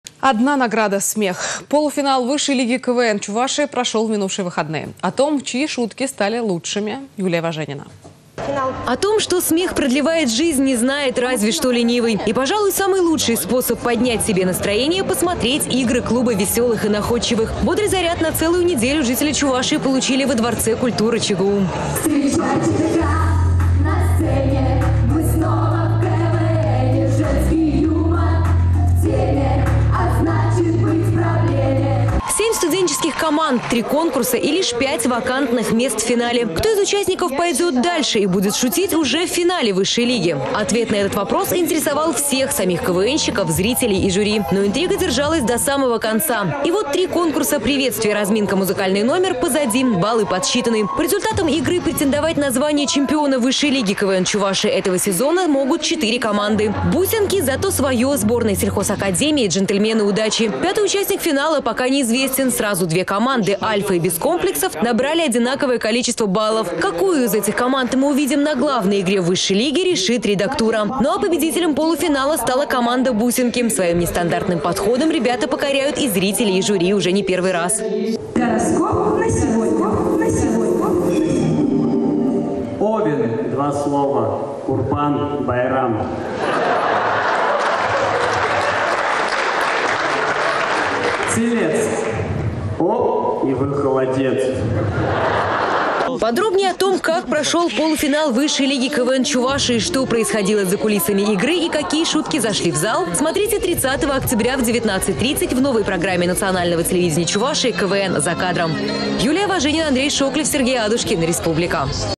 Полуфинал высшей лиги
Одна награда — смех. Полуфинал Высшей лиги КВН Чувашии прошел в минувшие выходные. О том, чьи шутки стали лучшими
- Юлия Важенина.